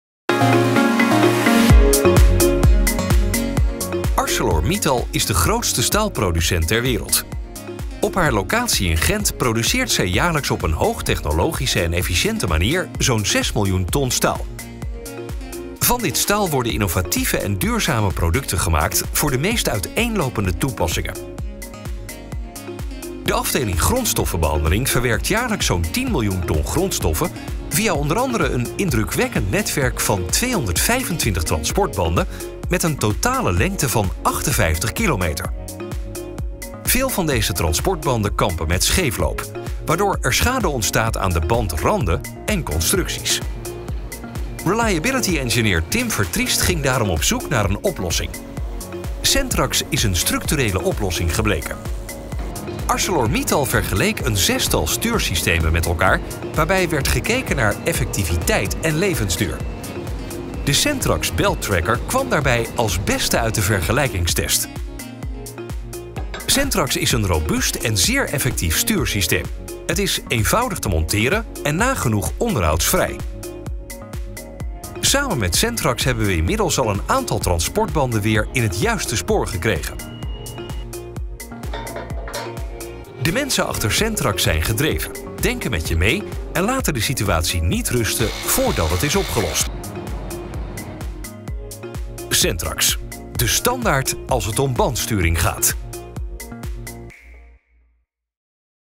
CenTrax houdt transportband ArcelorMittal Gent in het juiste spoor
ArcelorMittal Belgium kampte op haar locatie in Gent met fikse scheefloop bij 225 transportbanden met een totale lengte van 58 kilometer. Een storingsvrije transportband is voor een bedrijf als ArcelorMittal cruciaal. Het CenTrax stuursysteem is als beste uit hun vergelijking gekomen en een structurele oplossing gebleken.